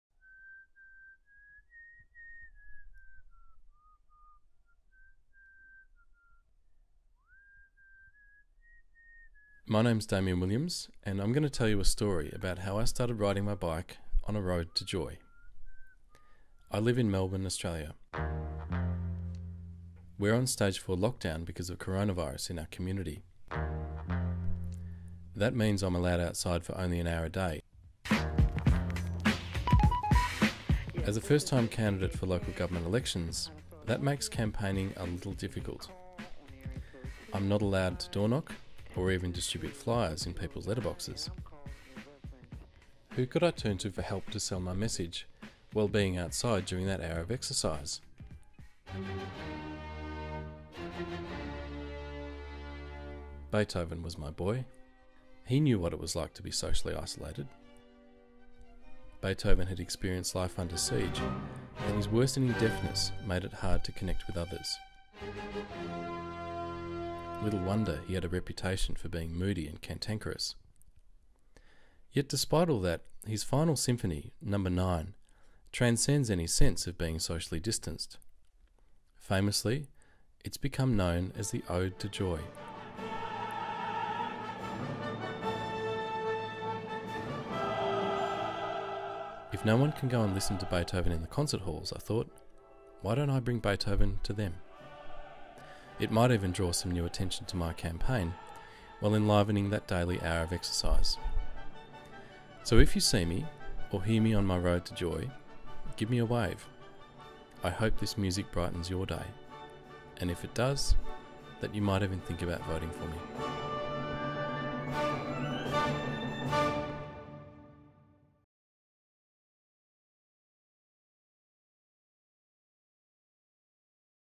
Road to Joy: a corona tale
This is a short film about calling on a famous composer to help you campaign for election when you're not allowed to campaign. It was made in Melbourne, Australia, in August 2020, when the city was on stage 4 lockdown.